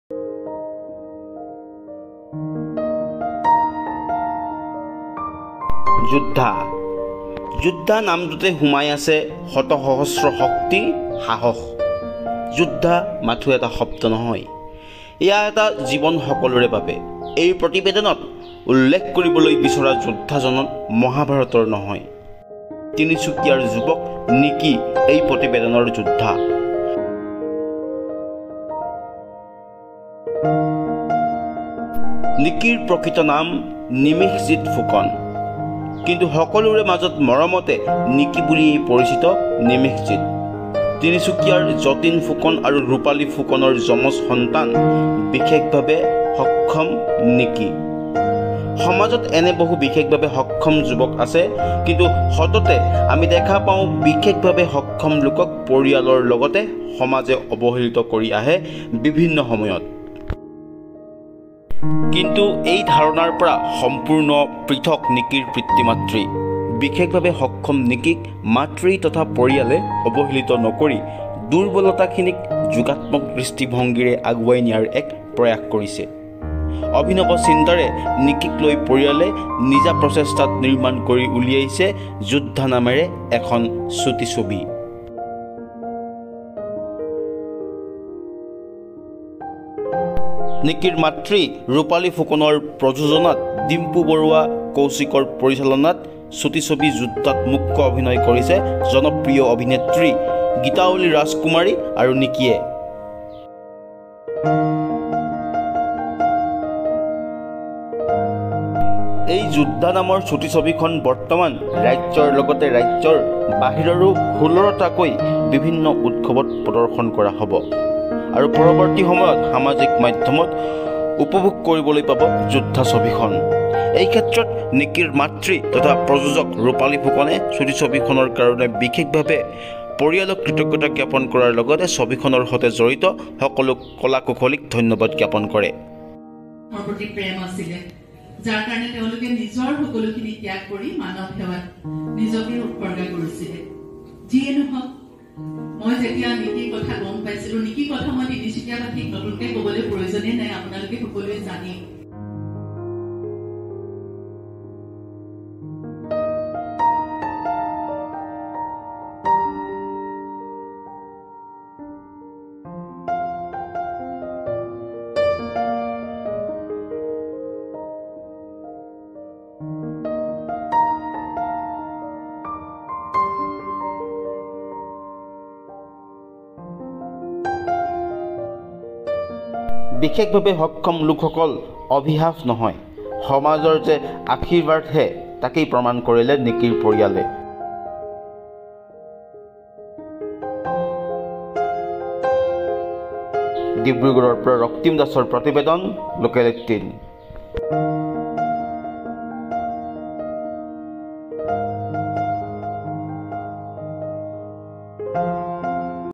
Tinsukia News : তিনিচুকীয়া জিলাৰ ‘যোদ্ধা’ নিকিক লৈ নিৰ্মাণ কৰা ছুটি ছবি “যোদ্ধা”ৰ শুভমুক্তি৷#local18
Tinsukia News : তিনিচুকীয়া জিলাৰ ‘যোদ্ধা’ নিকিক লৈ নিৰ্মাণ কৰা ছুটি ছবি “যোদ্ধা”ৰ শুভমুক্তি৷#local18#news18assamne